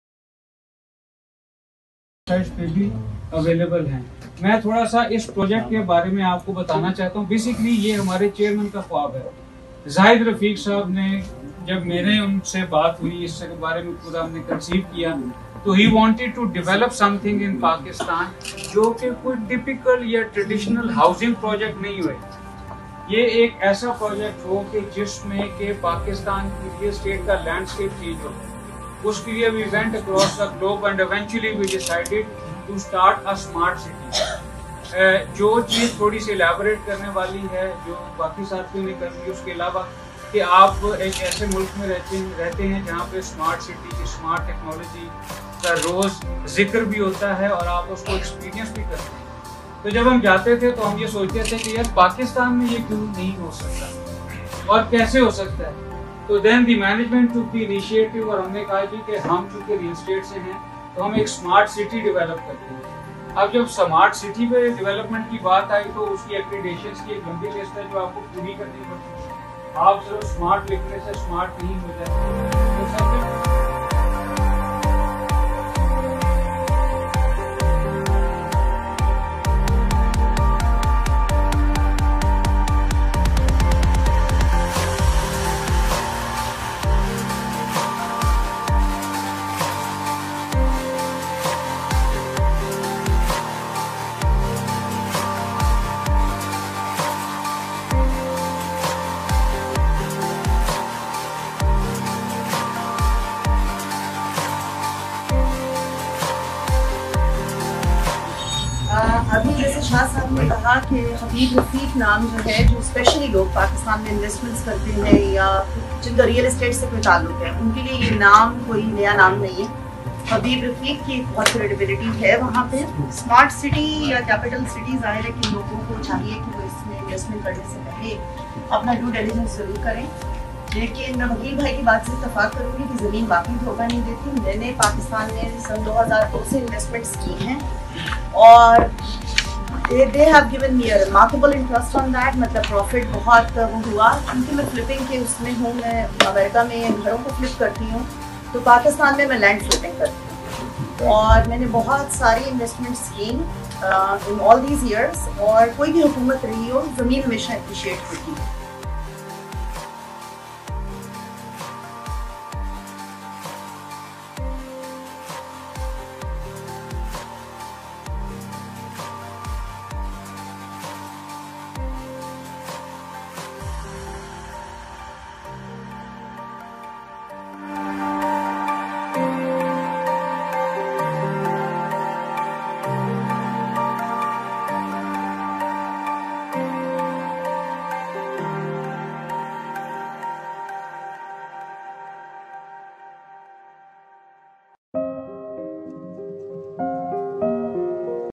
Open house event in New York - Capital Smart City
By the grace of Almighty Allah, guidance and support of all our valuable team members, the open house event in New York Nov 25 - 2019 was a great success. More than 125 participants from all walks of life were there. Our project was applauded and people showed keen interest for investment in our Capital Smart City. Our realtors in New York worked really hard to gather the right audience. Thank you everyone!